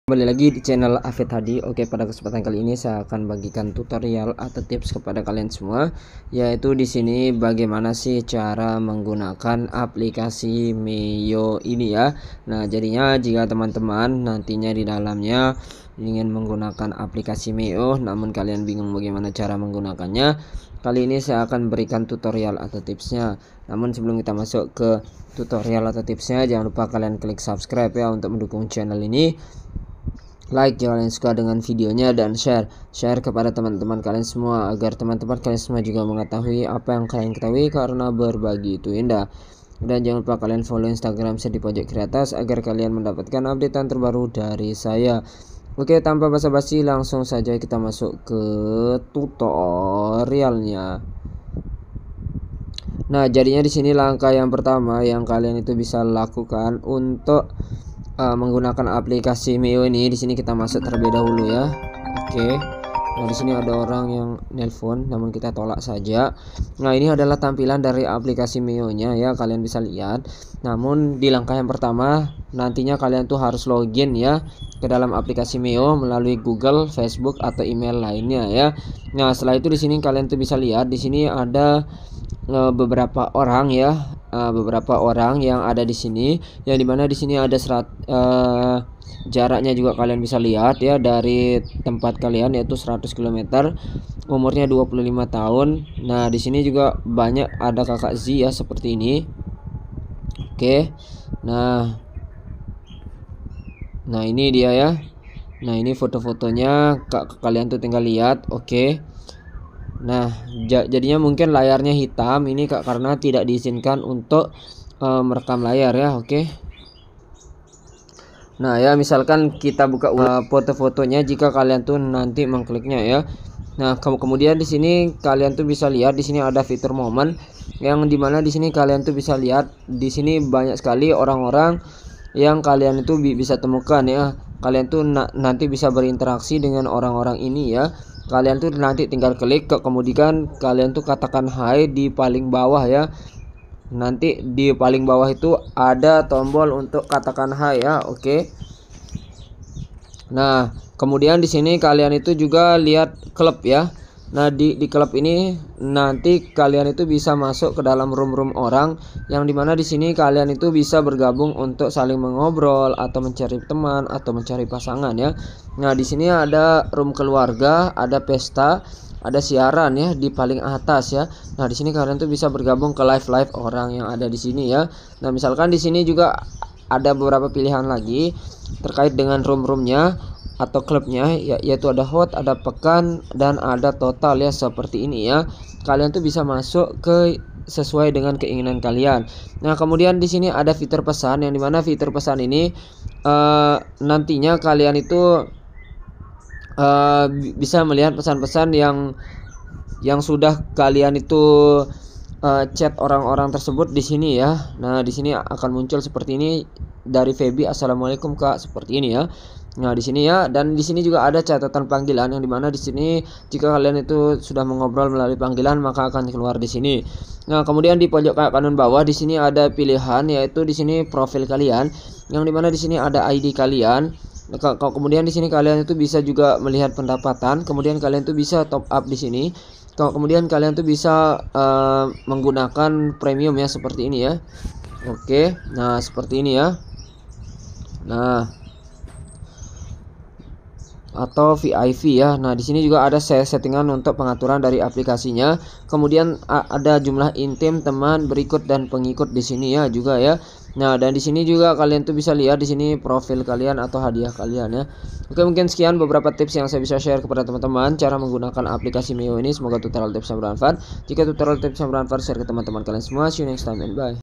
Cara Menggunakan Aplikasi Meyo | Cara Pakai Aplikasi Meyo
Assalamualaikum warahmatullahi wabarakatu ini saya akan memberikan informasi yaitu bagaimana Cara Menggunakan Aplikasi Meyo semoga video ini bermanfaat bagi anda semua.